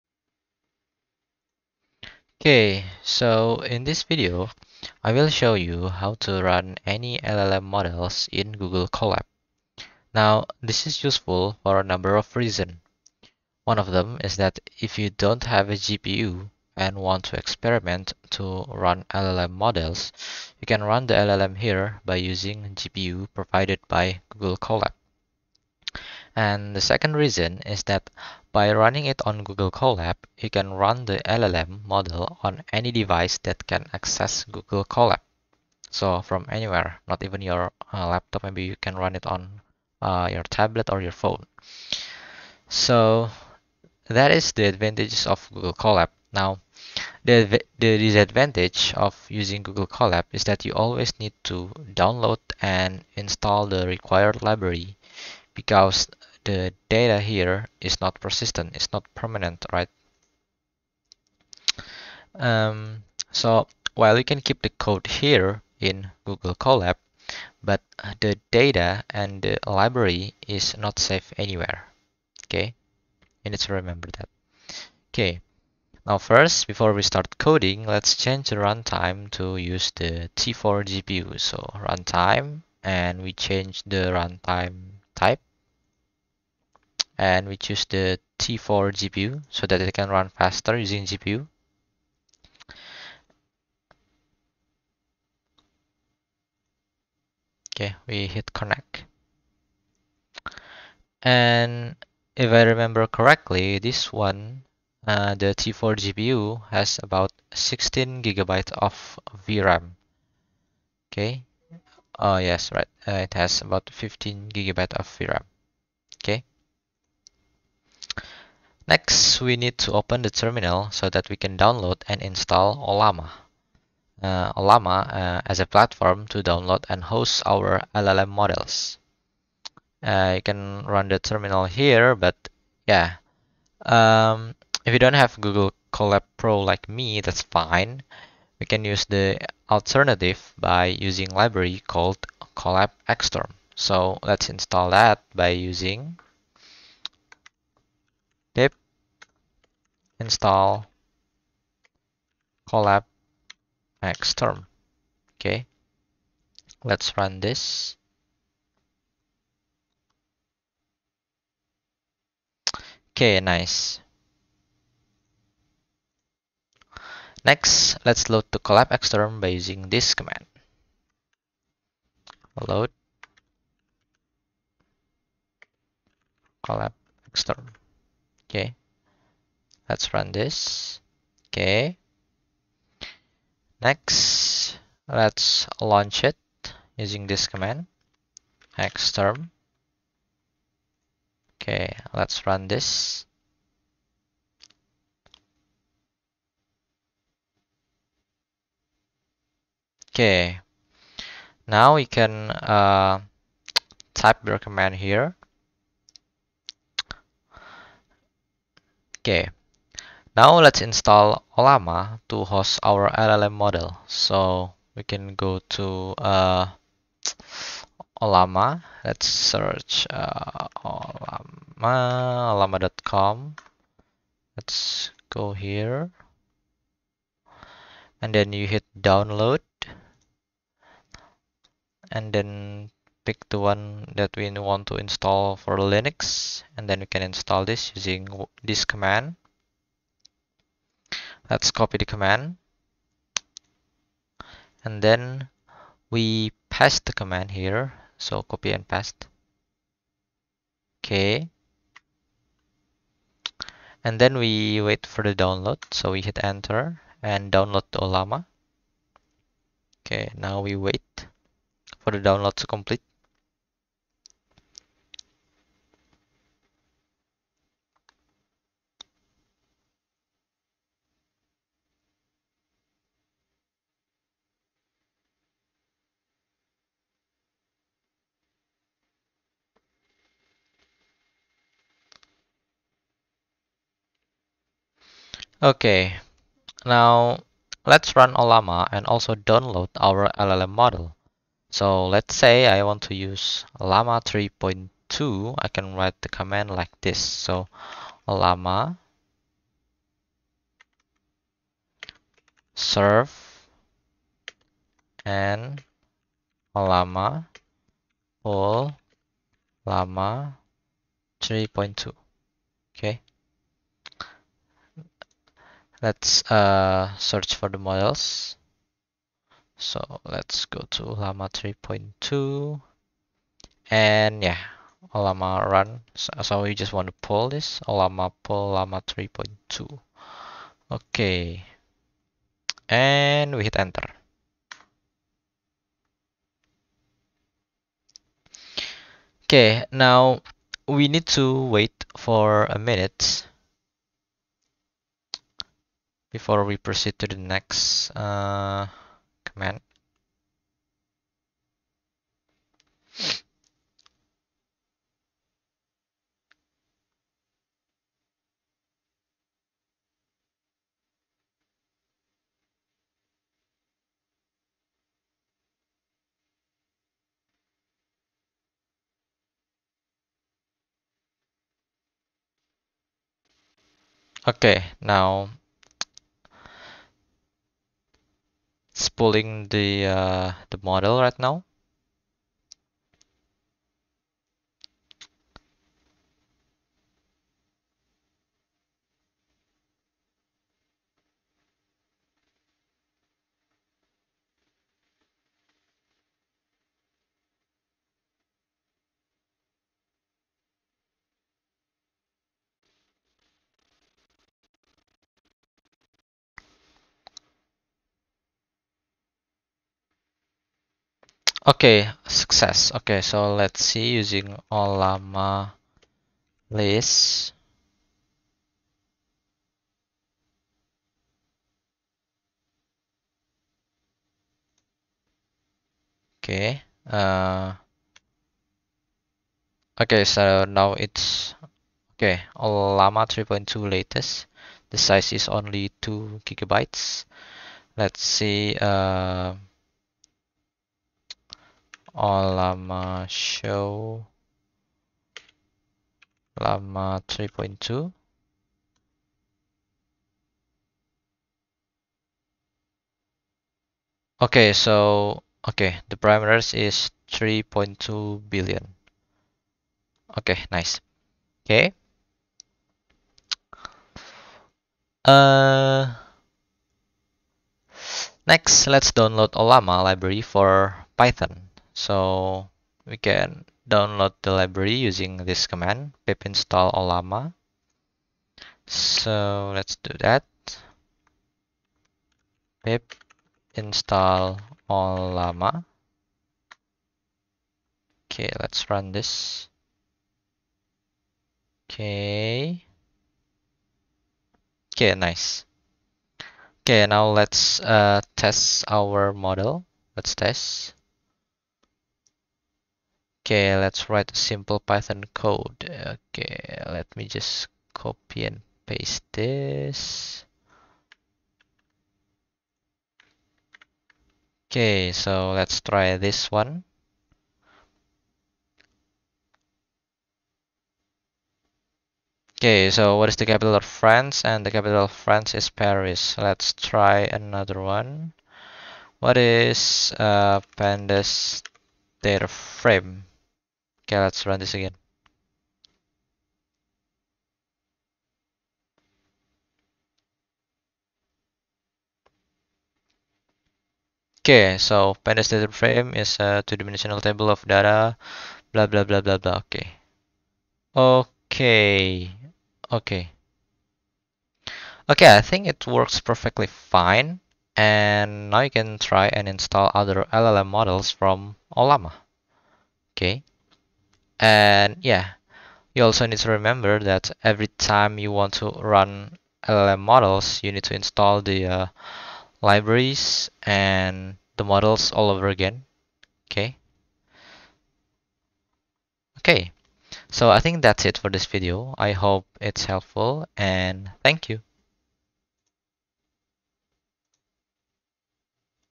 How to Run Ollama LLM Model on Google Colab
In today's video, I'll guide you through the process of running any LLM (Large Language Model) on Google Colab. This is perfect for those of you who don’t have a GPU and want to experiment with running LLM models. Google Colab offers free GPU resources, and I'll show you how to make the most out of it! 🚀

What You’ll Learn:

- Why Google Colab is a great platform for running LLM models.
- Step-by-step setup to run the T4 GPU for faster performance.
- Installing and using Ollama to host and manage LLM models like Llama 3.2.
- How to set up and use the Colab terminal with colab-xterm for a seamless experience.
- Tips for running Python code to interact with LLM models.

Cara Menjalankan LLM di Google Colab - Panduan Langkah Demi Langkah

Di video kali ini, saya akan menunjukkan cara menjalankan model LLM (Large Language Model) di Google Colab. Ini sangat cocok bagi kalian yang tidak memiliki GPU namun ingin mencoba menjalankan model LLM. Google Colab menyediakan GPU gratis, dan saya akan menunjukkan cara memanfaatkannya secara maksimal! 🚀

Yang Akan Kamu Pelajari:

- Mengapa Google Colab adalah platform yang bagus untuk menjalankan model LLM.
- Langkah-langkah untuk mengatur GPU T4 agar mendapatkan performa yang lebih cepat.
- Cara menginstal dan menggunakan Ollama untuk mengelola model LLM seperti Llama 3.2.
- Cara mengatur dan menggunakan terminal Colab dengan colab-xterm untuk pengalaman yang lancar.
- Tips untuk menjalankan kode Python agar dapat berinteraksi dengan model LLM.